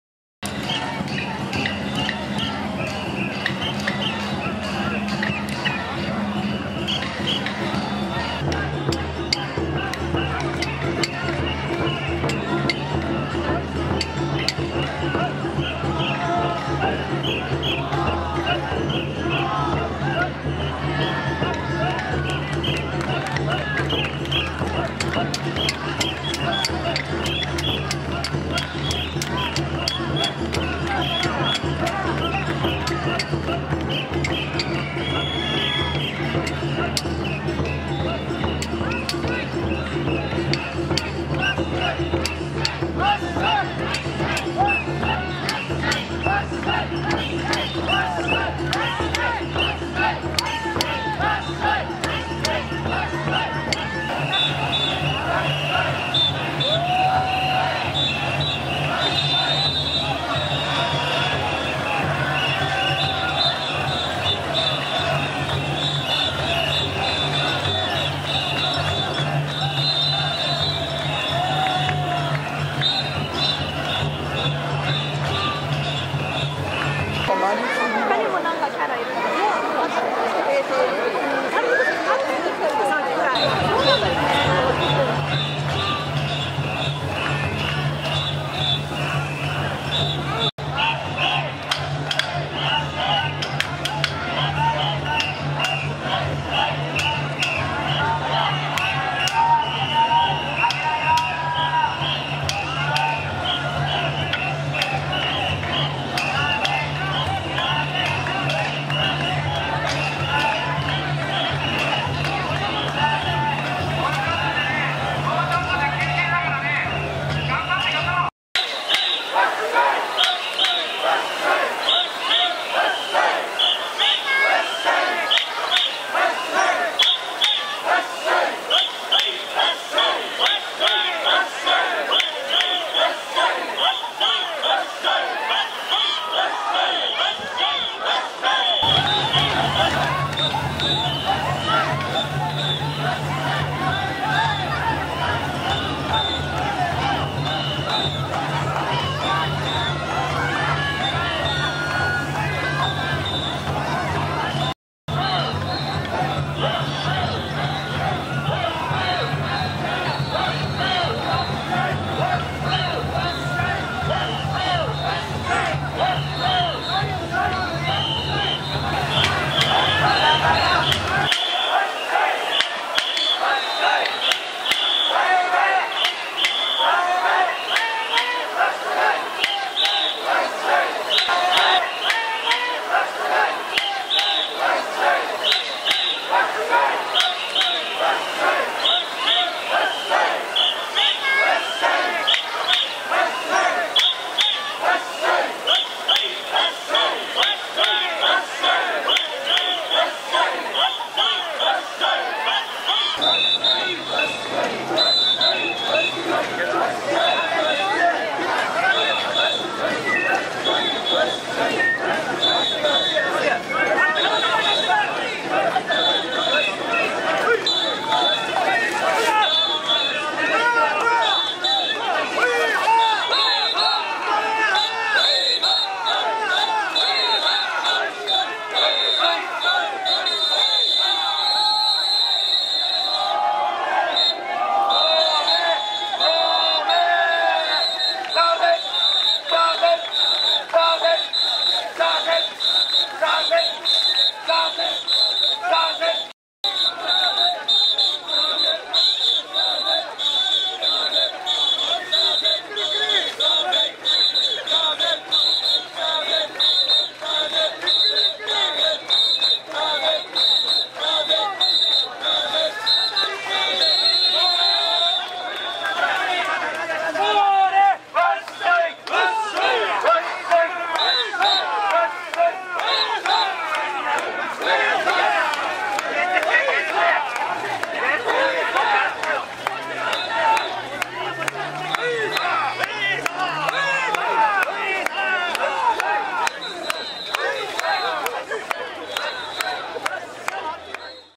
チコちゃんも登場、こども神輿連合の富岡八幡宮
２０１９年８月１１日　江東区富岡八幡宮祭礼　今年は子供みこし連合です。町内神輿は午後でした。